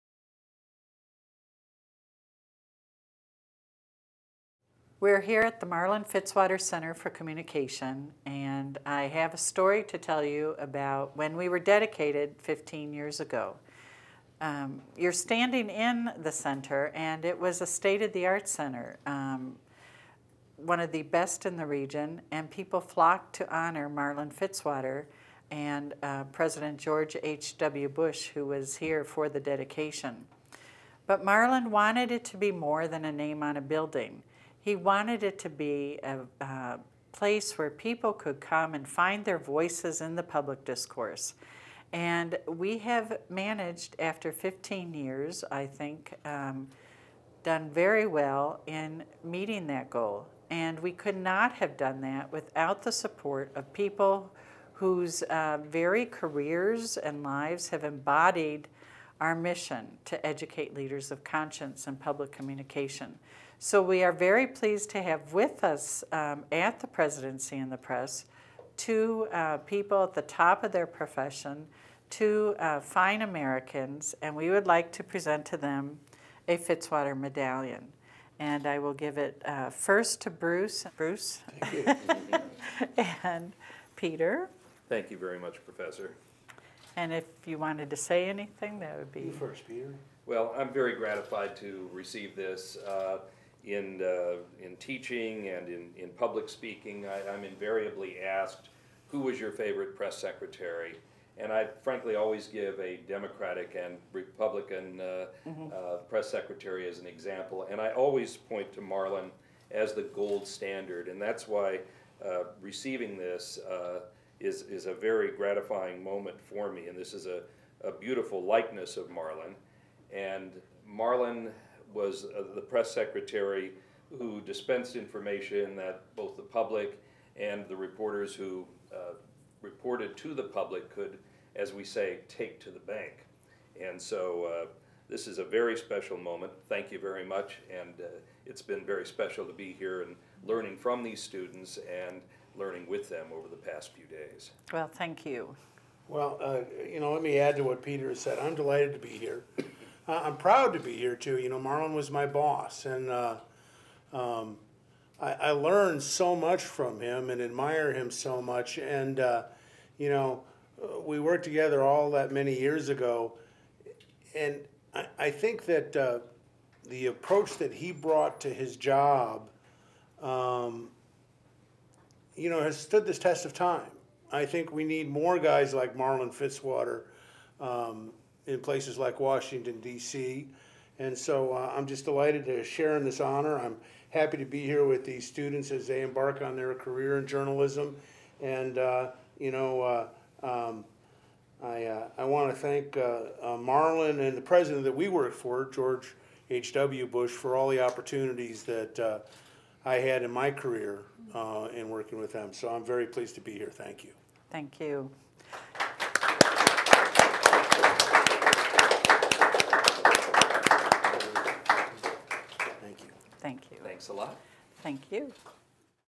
Fitzwater Medallion Presentation
During The Presidency and the Press, Doctor Kristen Nevious presented Peter Maer, Veteran CBS Radio Correspondent for the White House, and Bruce Zanca, White House Press Secretary for three Presidents, with the Fitzwater Medallion.  This took place in front of the portrait of Marlin FItzwater, in the Marlin FItzwater Center for Communication.